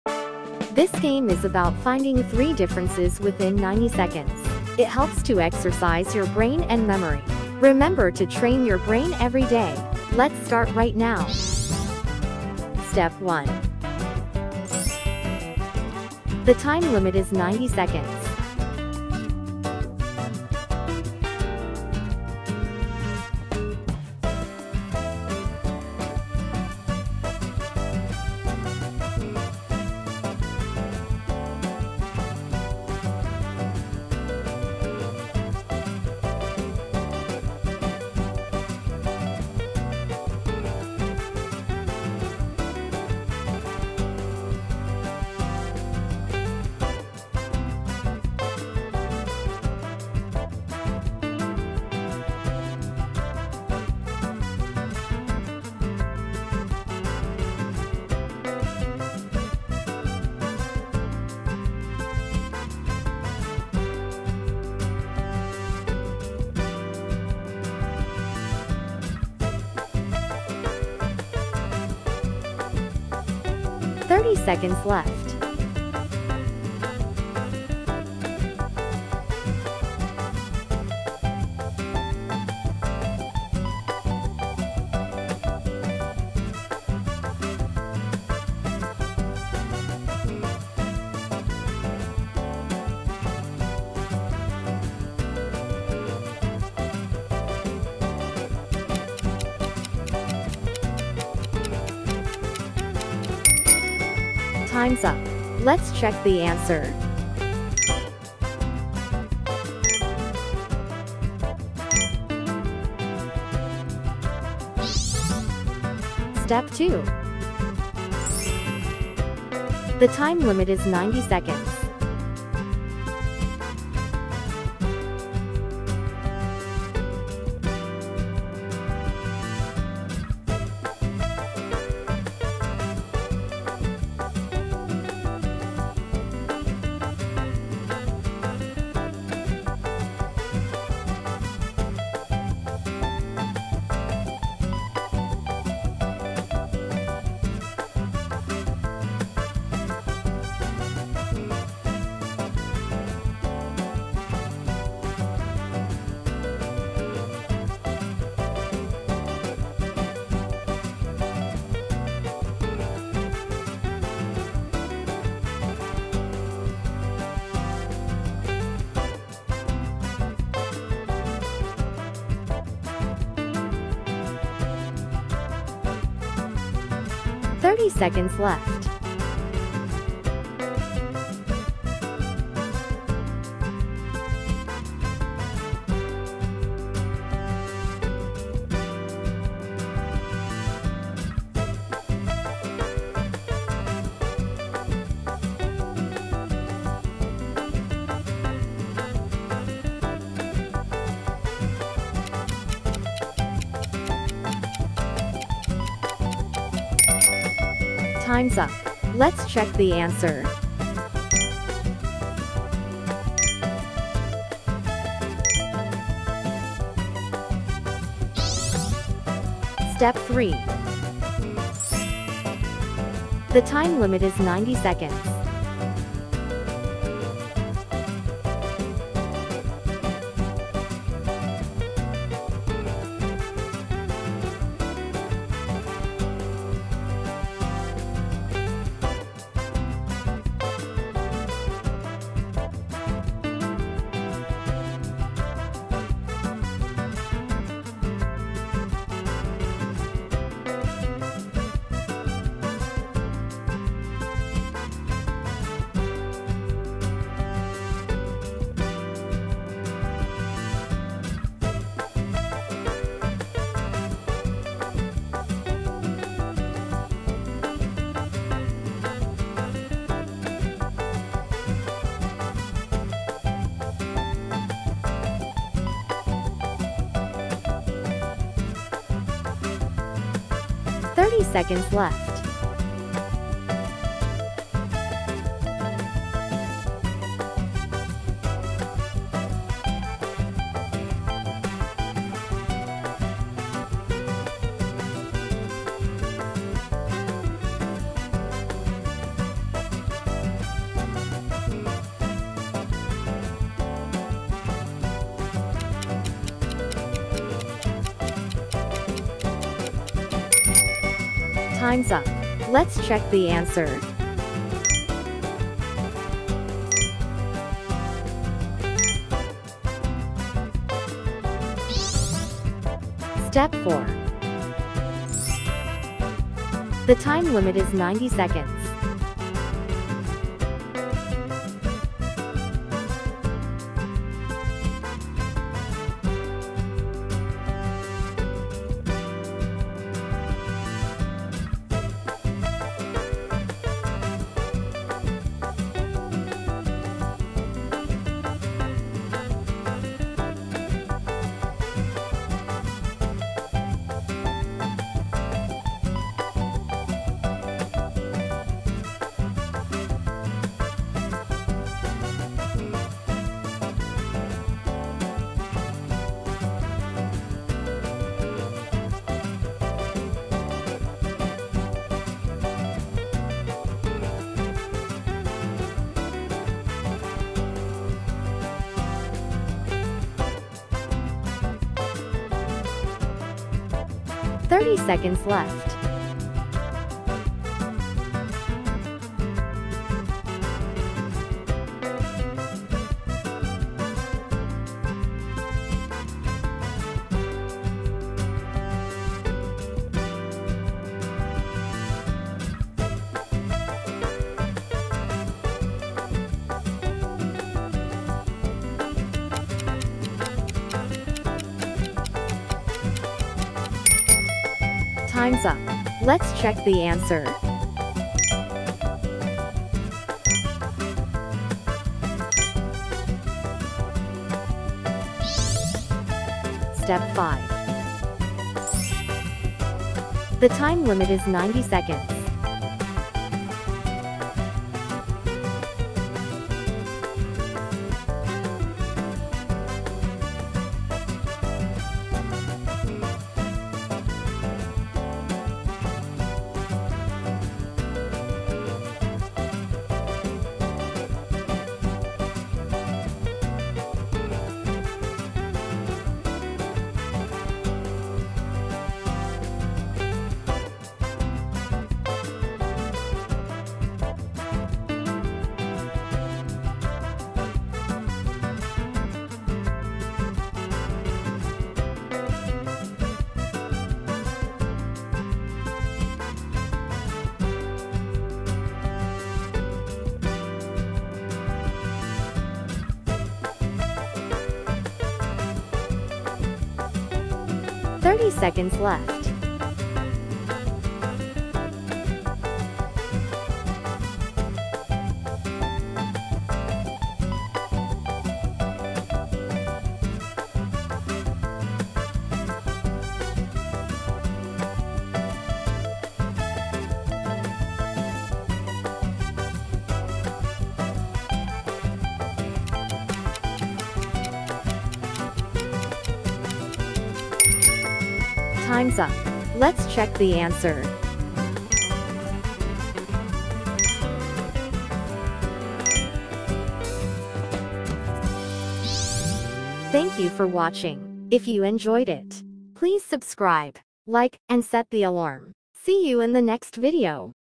【Spot the Difference Game】 Only Genius Can Find ! Brain Game !
Welcome to the Catch Panda Channel 💡
this game requires players to identify three differences between two images within 90 seconds. 🔍

00:00 Panda Opening Greetings
00:15 STEP 1
01:45 STEP 1 Answer
01:59 STEP 2
03:28 STEP 2 Answer
03:42 STEP 3
05:12 STEP 3 Answer
05:26 STEP 4
06:55 STEP 4 Answer
07:09 STEP 5
08:39 STEP 5 Answer
08:49 Panda Ending Greetings